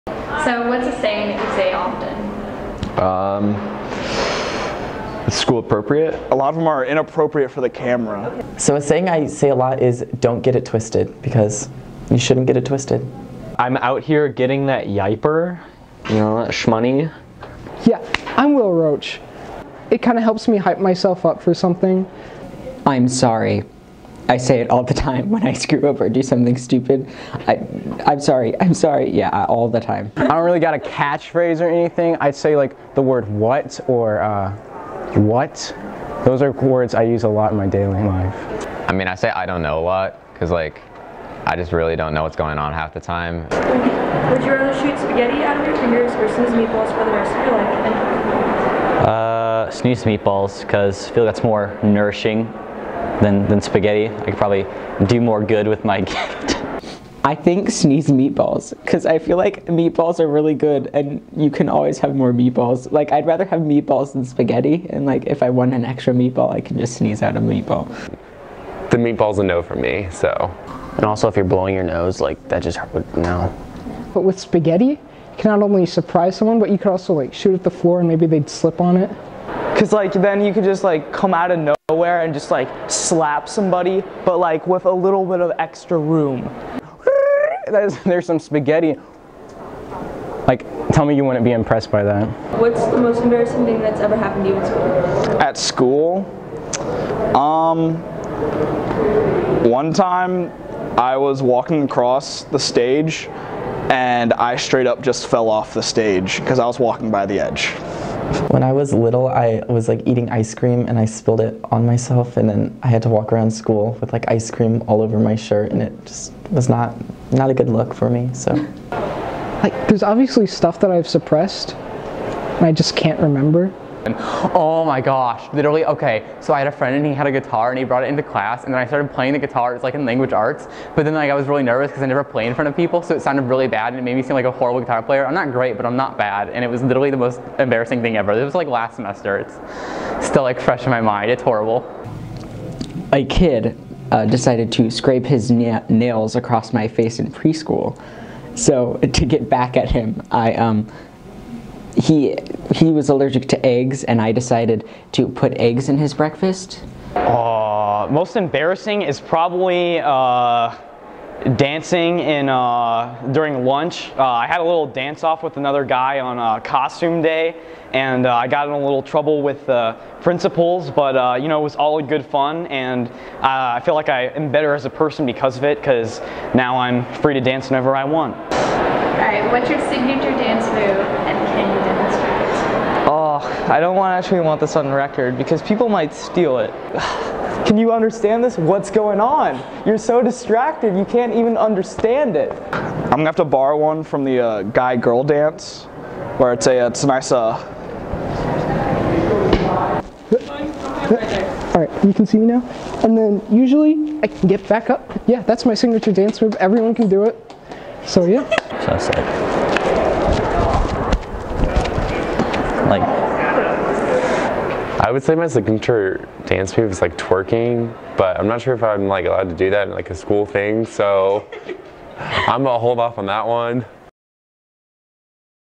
MHSNews | Mr. Mustang 2018-2019 Contestant Interviews
Mr. Mustang is a competition in which participating Marquette clubs, sports, and classes submit a male representative to perform in several events. Mr. Mustang is a Prom fundraiser.  MHSNews interviewed this year's contestants.
Video by associate producer staff reporter Jen Bosche.